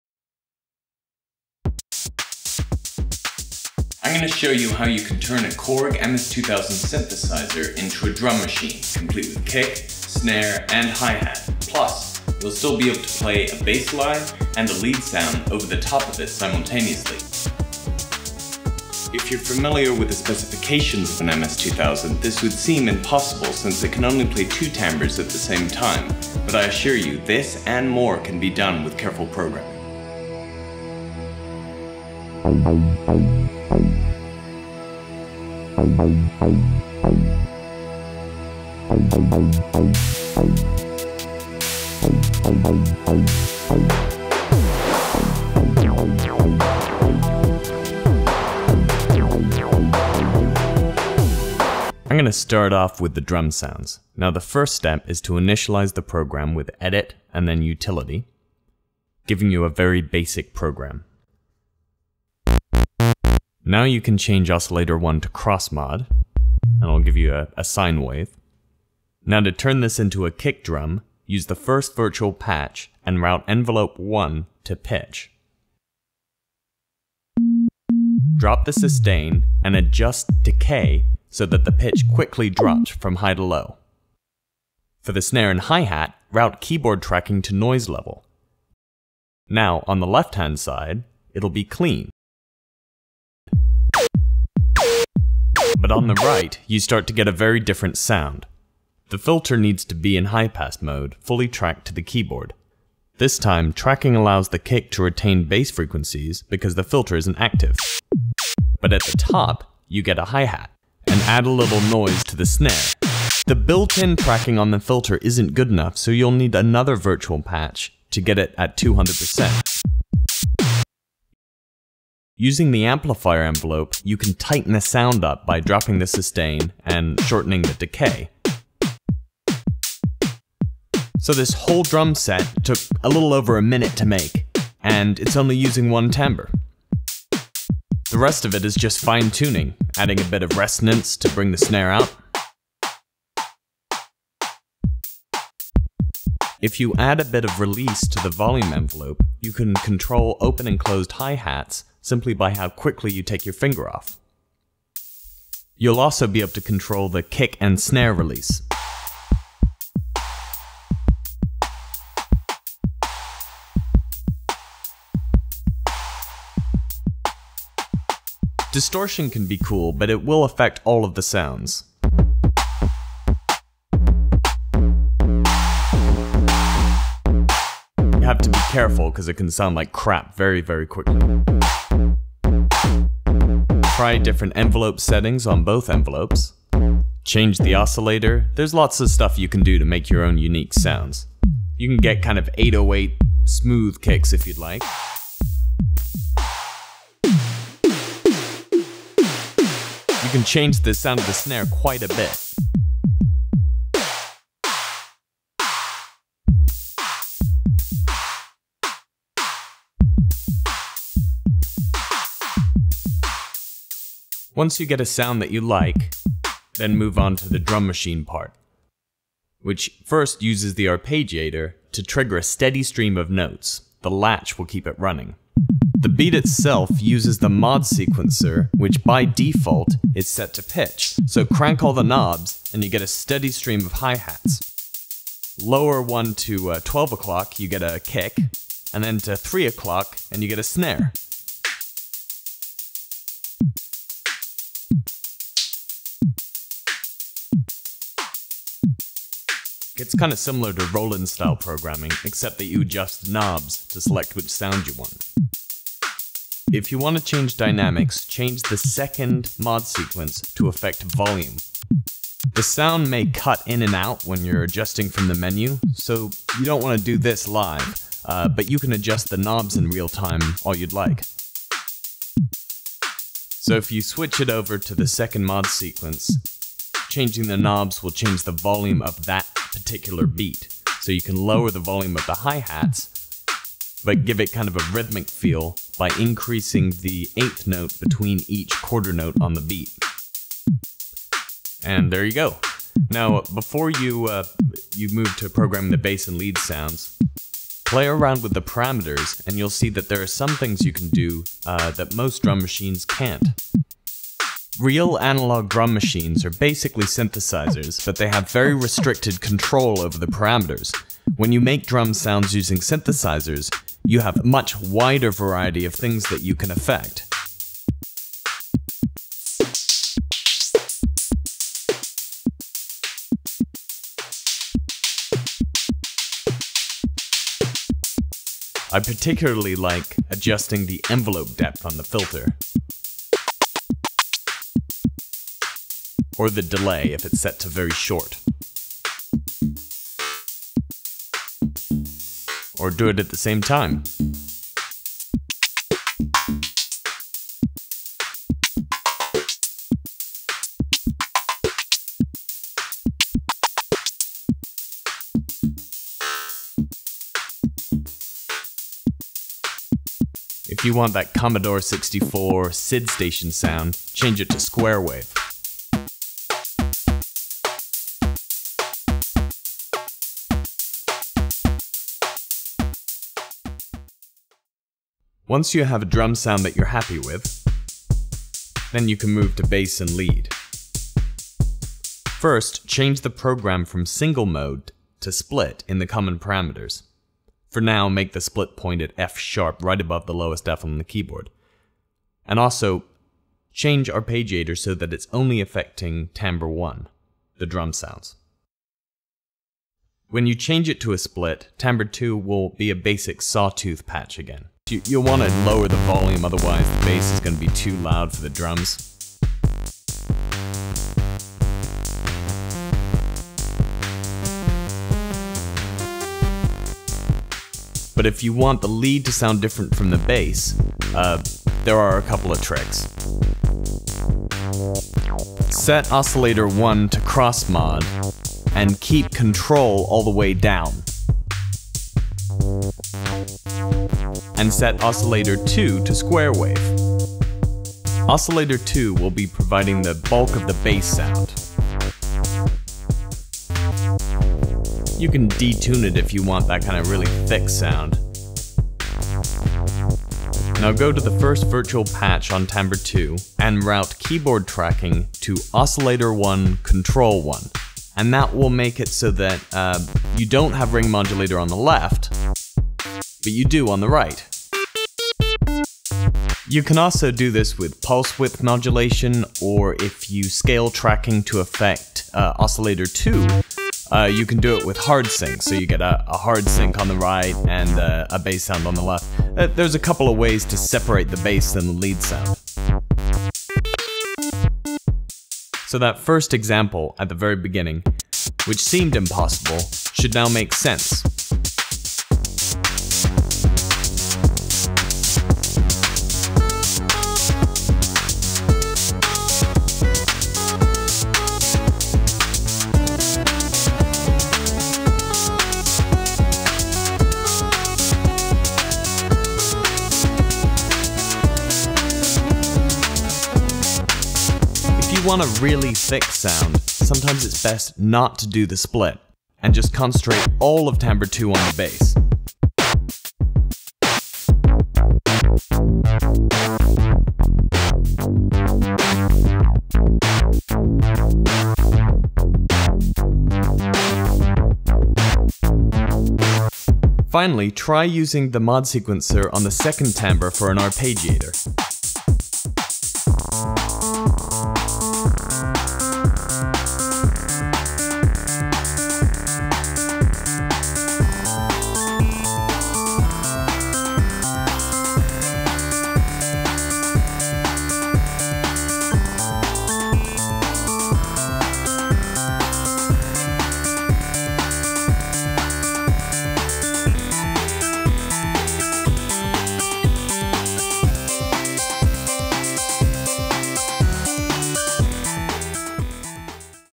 Inside Synthesis EP1: Turn an analog modelling synth into a drum machine (2007)
This is the first episode of Inside Synthesis, updated higher  quality video, audio, and graphics. It's a tutorial on how program a Korg MS-2000 to function as a 16 step drum machine, while still being able to play bass and lead lines over it.